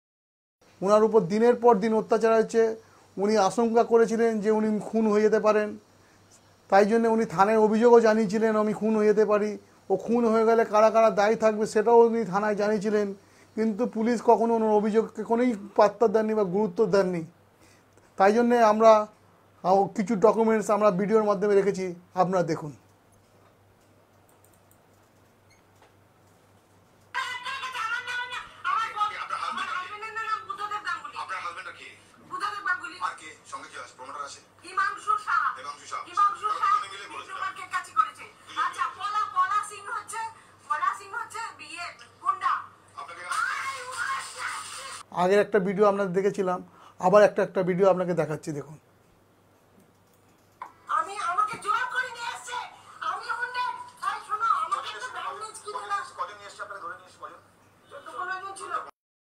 Sunanda's neighbour exposes two videos where she had expressed her apprehension of being k
Sunanda's neighbour exposes two videos where she had expressed her apprehension of being killing long ago